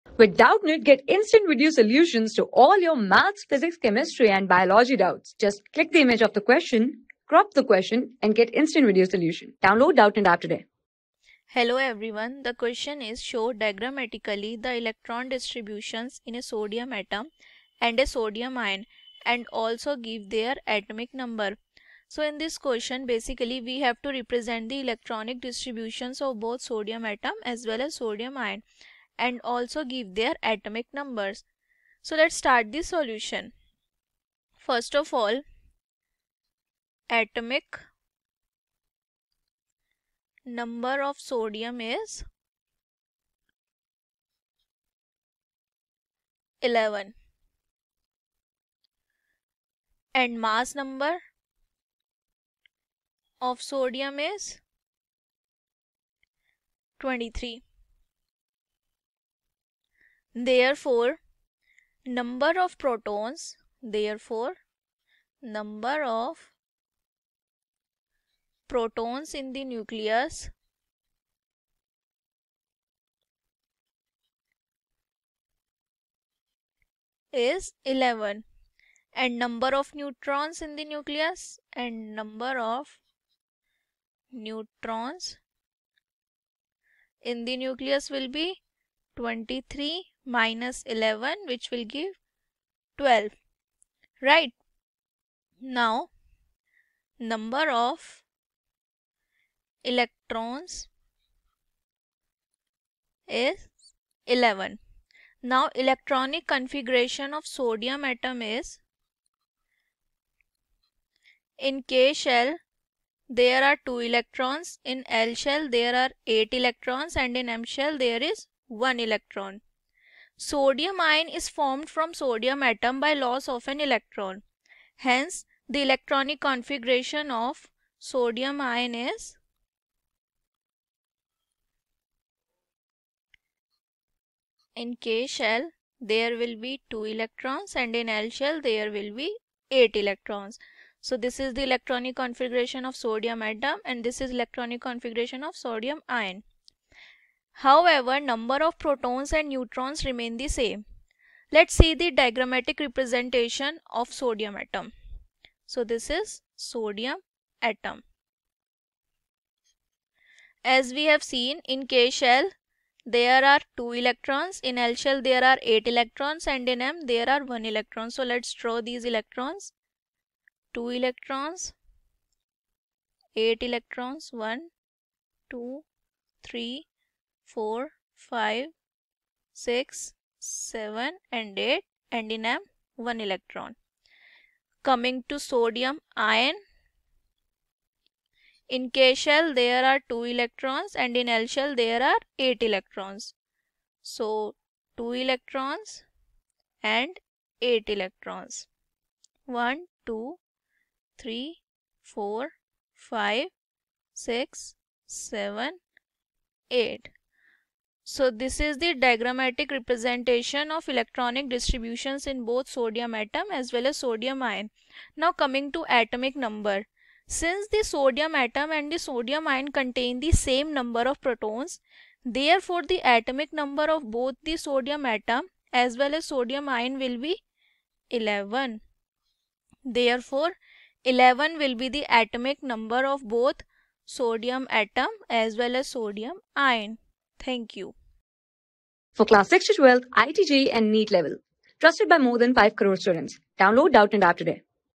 Show diagramatically the electron distributions in a sodium atom and a sodium ion and also give ...
Show diagramatically the electron distributions in a sodium atom and a sodium ion and also give their atomic number.
Class: 9
Subject: CHEMISTRY
Chapter: STRUCTURE OF THE ATOM
Board:CBSE

👉 Have Any Query? Ask Us.